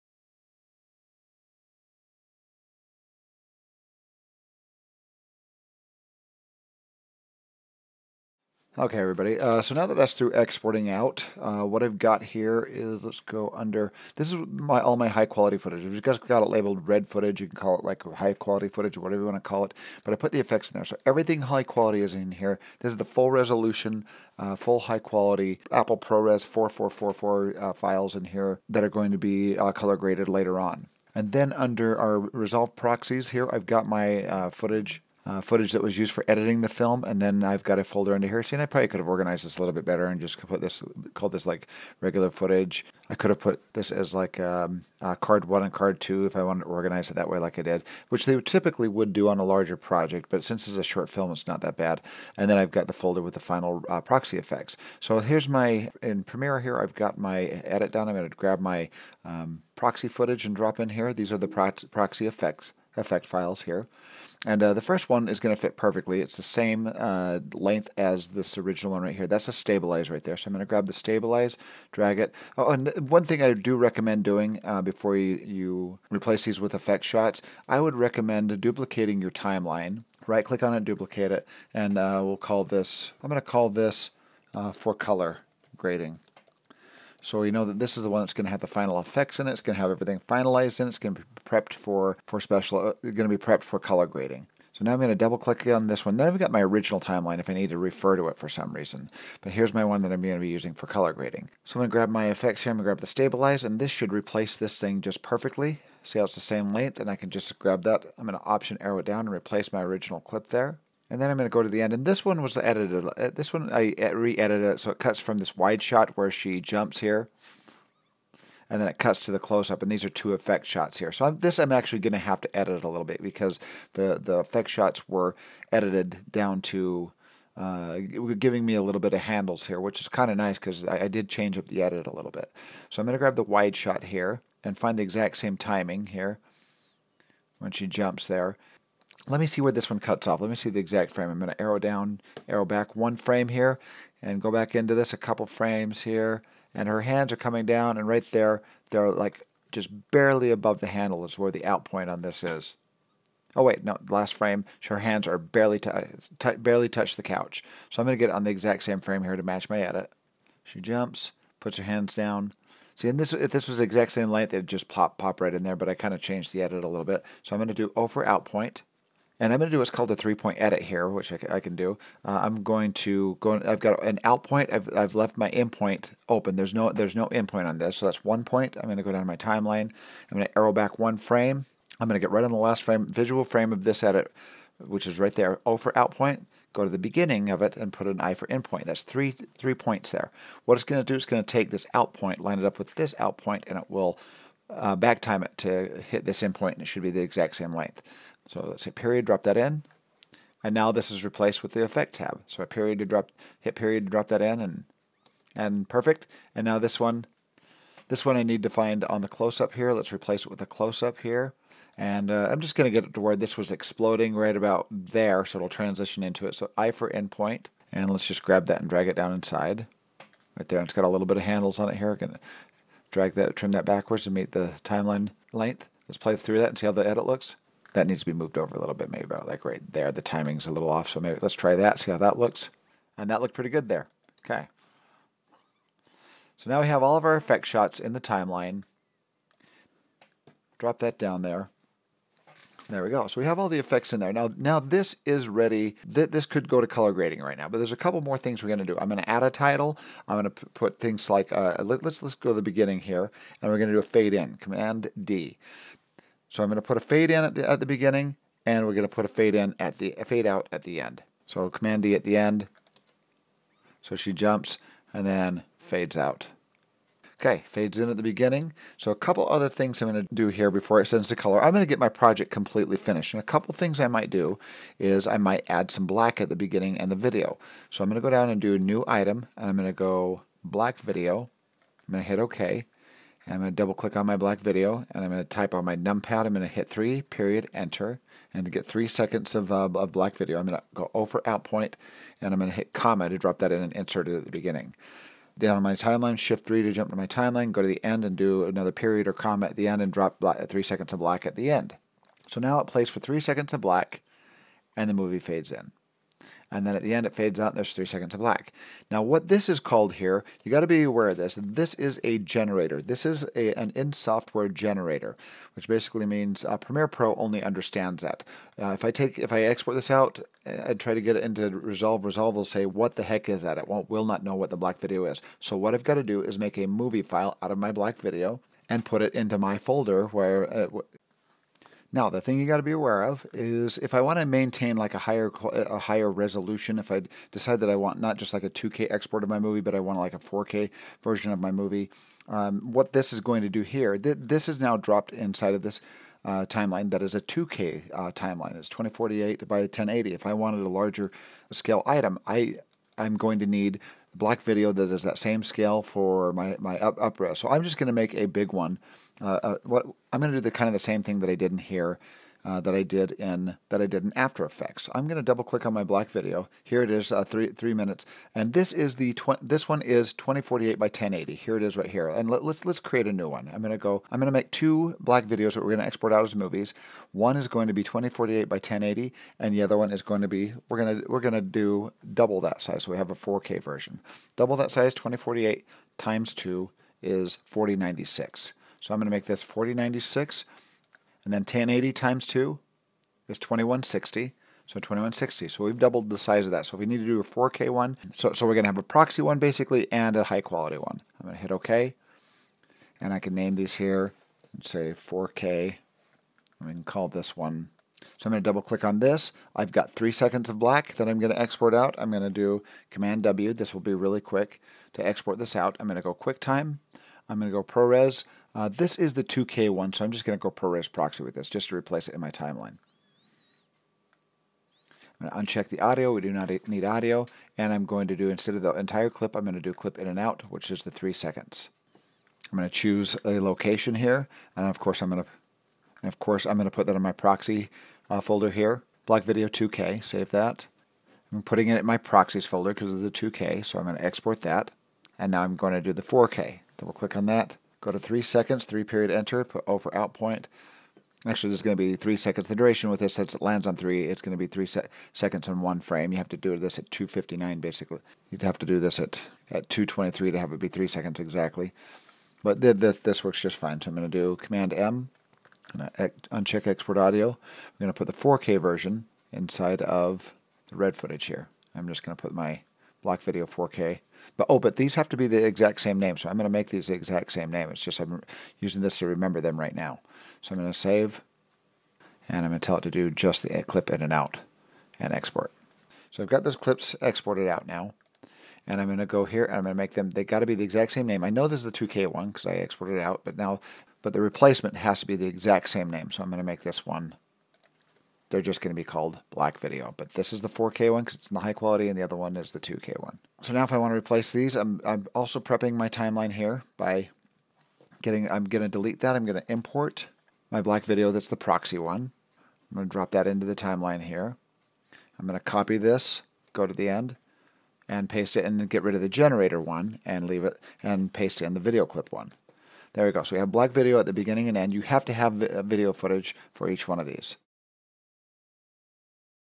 E3 - How to Color Grade: Davinci Resolve: Sending to Resolve Pt. 2- Exporting Effects and Generators
Create proxies in Davinci Resolve, then take the footage to Premiere Pro for editing. Then take the edit to Resolve for color grading.